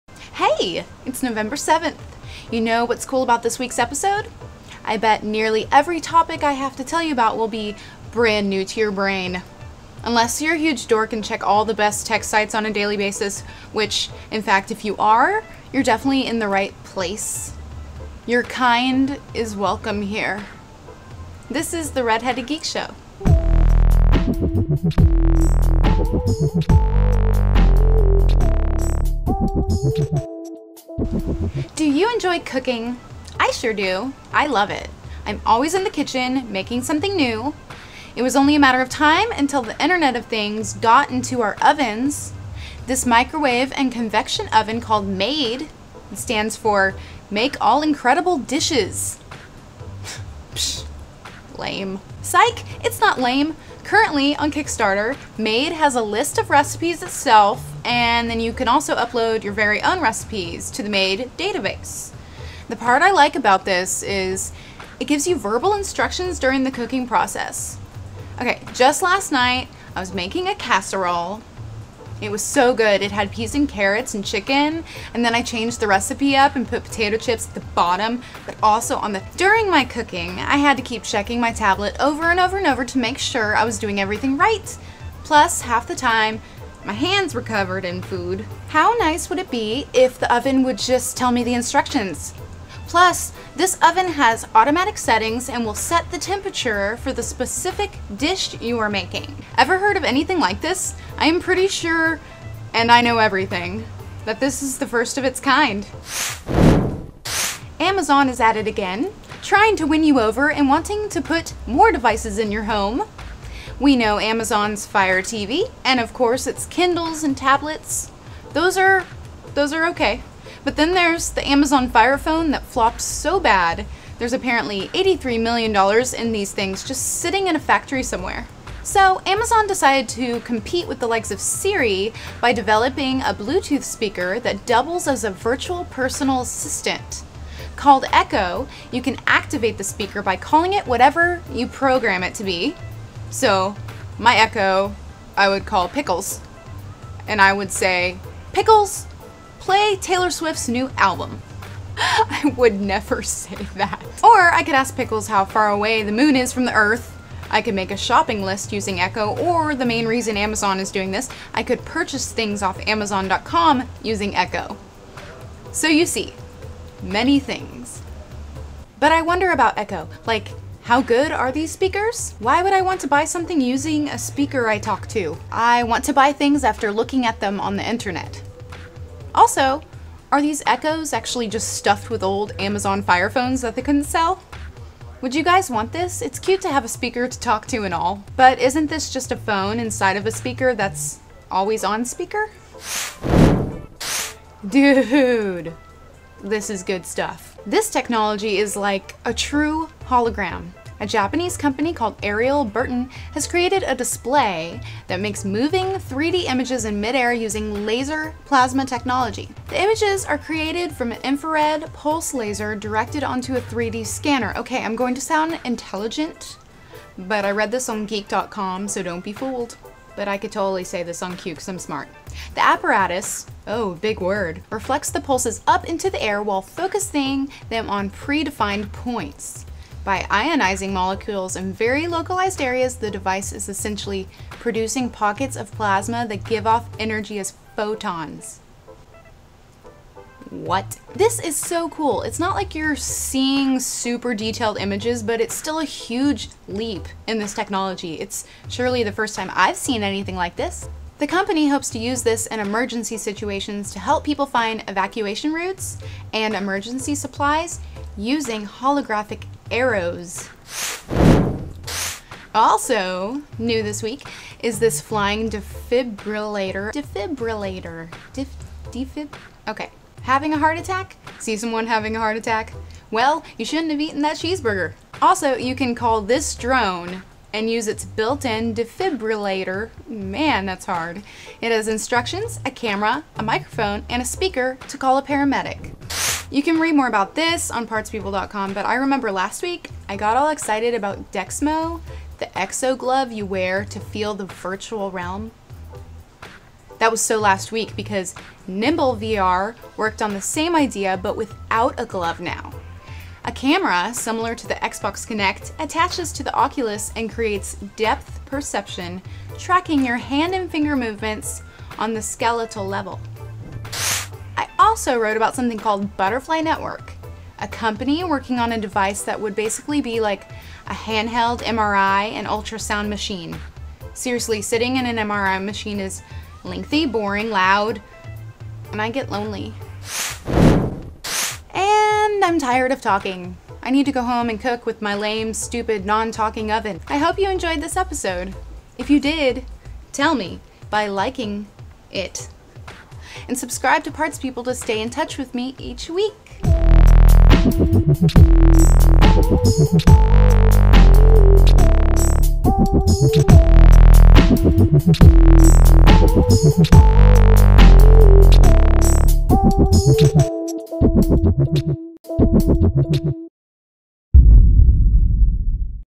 E 85- Echo, MAID, Nimble VR, Butterfly Network | Red Headed Geek Show
MAID is the smartest oven ever. Thanks to voice recognition, it gives you verbal instructions during the cooking process.

Amazon decided to compete with the likes of Siri by developing a Bluetooth speaker, called Echo, that doubles as a virtual personal assistant.

A Japanese company called Aerial Burton has created a display that makes moving 3D images in mid air using laser plasma technology.

Nimble Sense uses a camera similar to the Xbox Kinect to create depth perception in VR, tracking your hand and finger movements on the skeletal level.